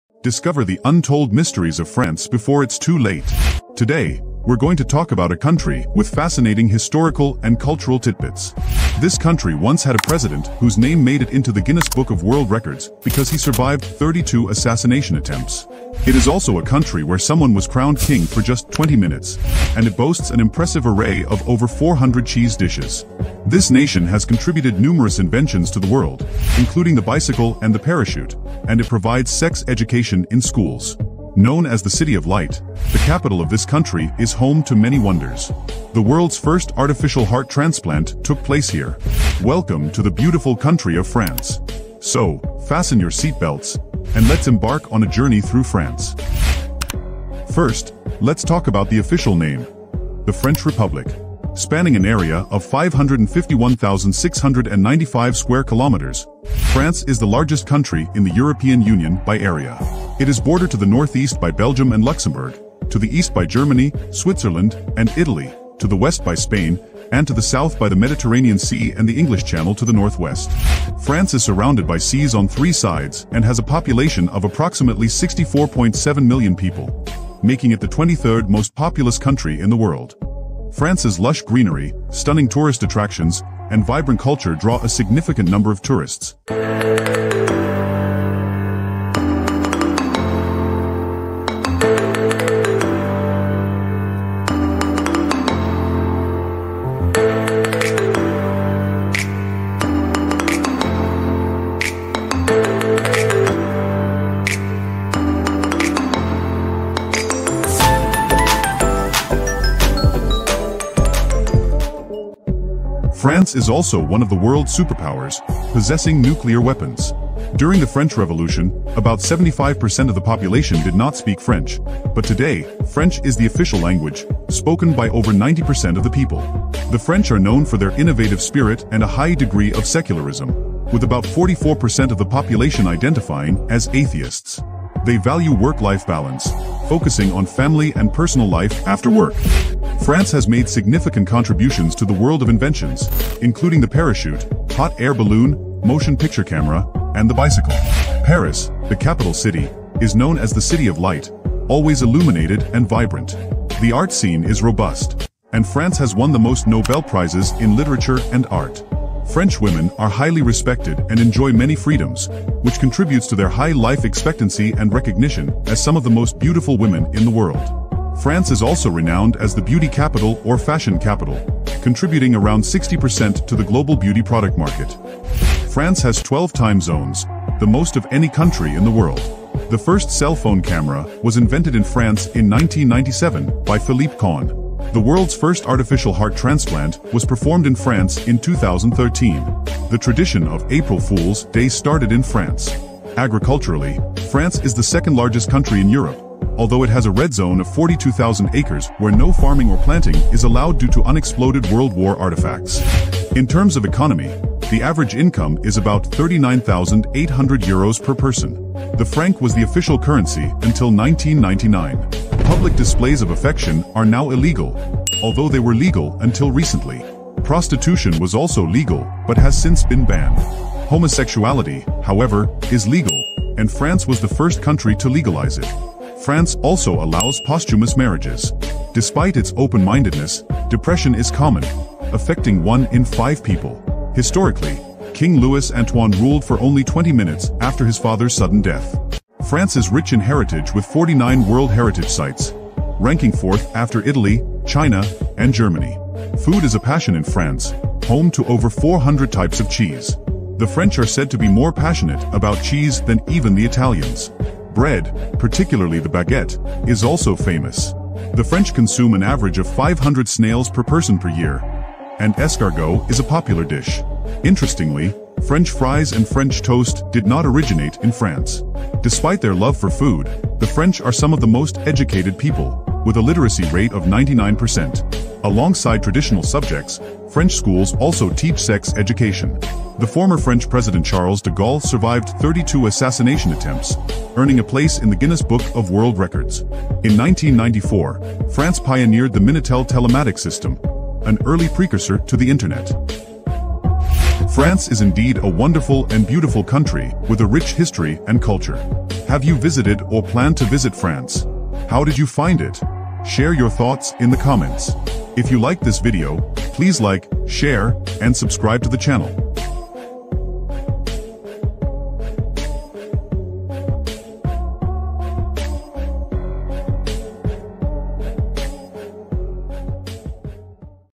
Discover the Untold Mysteries of France Before It’s Too Late!
Welcome to an exciting journey through the beautiful country of France! 🇫🇷 Discover fascinating historical and cultural facts about France, from surviving multiple assassination attempts 🛡️ to having a king for just 20 minutes 👑. Explore the rich heritage, impressive inventions, and delicious cuisine 🍷🧀 that make France a unique and captivating destination. Learn about the vibrant art scene 🎨, innovative spirit 💡, and interesting traditions that shape the French way of life. Join us as we delve into the wonders of France and share your experiences in the comments below. 👍📢✨